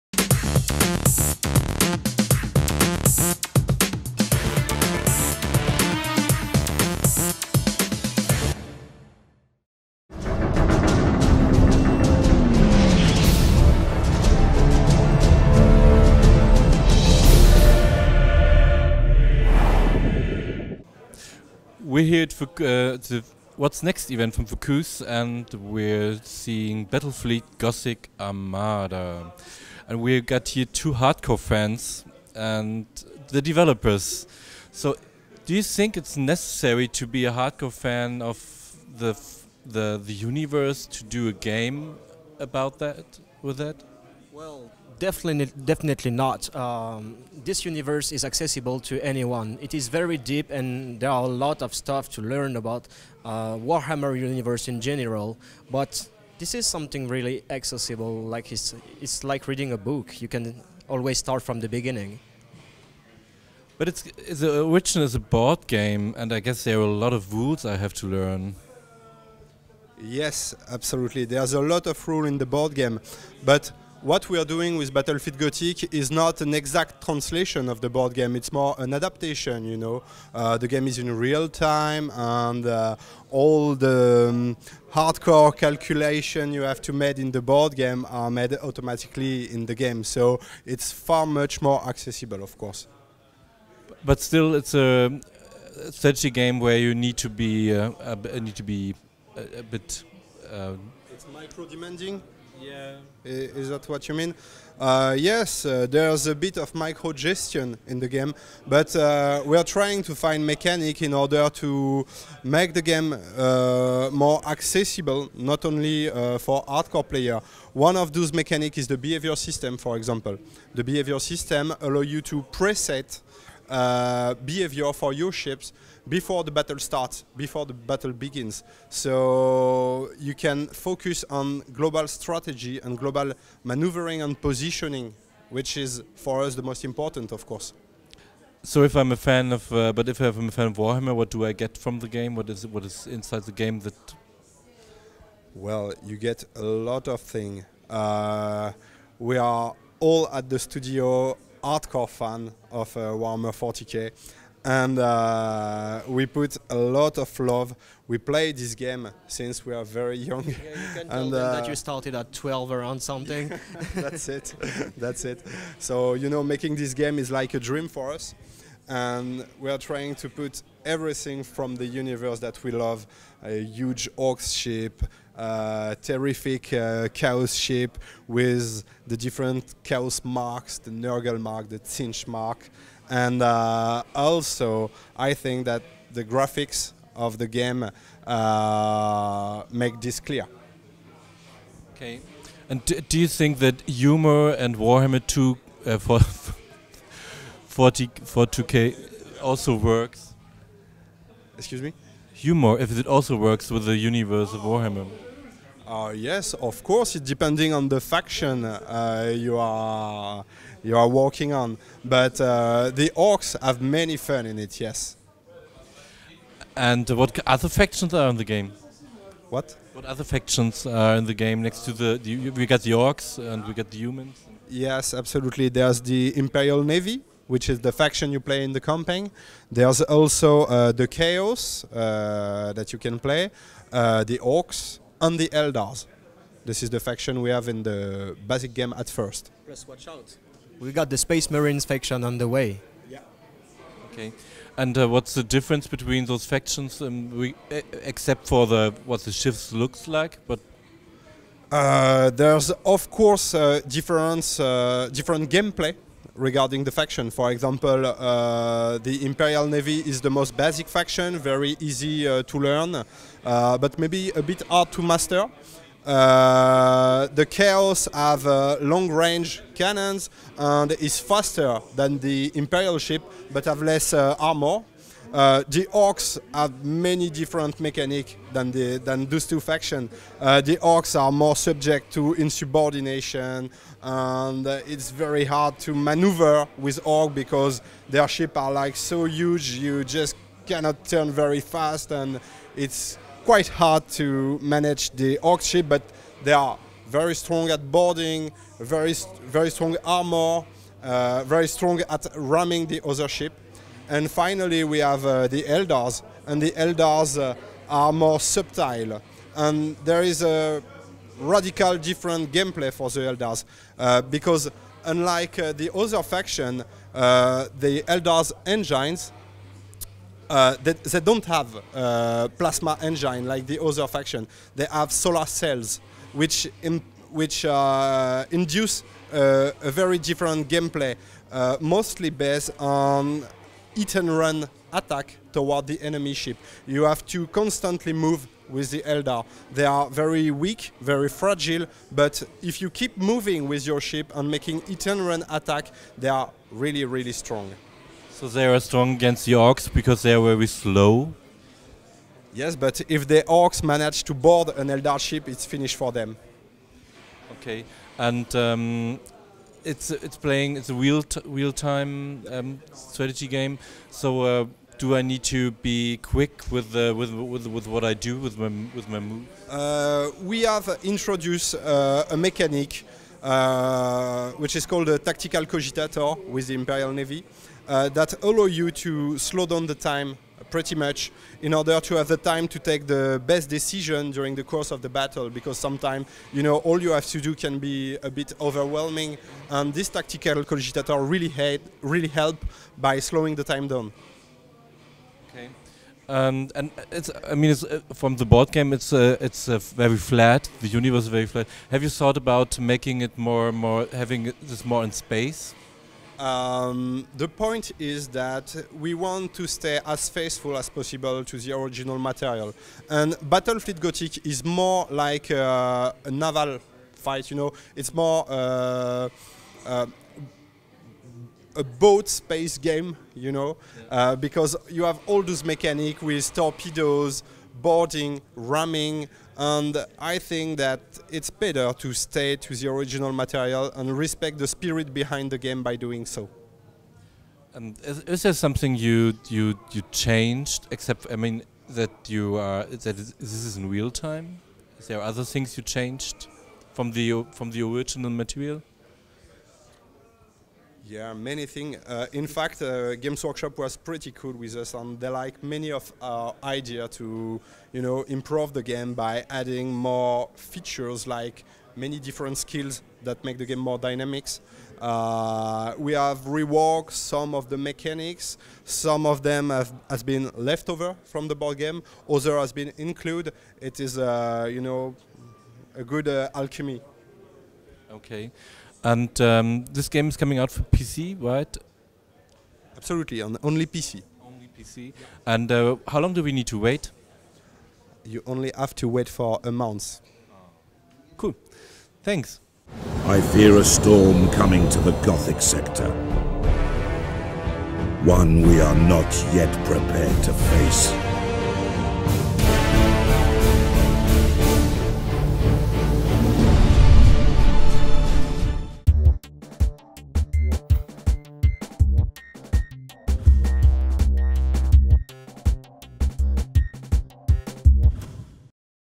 Battlefleet Gothic: Armada - Maxime Josse och Romain Clavier Interview
Maxime Josse and Romain Clavier talk to Gamereactor about what fans can expect from Battlefleet Gothic: Armada, which is set to launch later this month.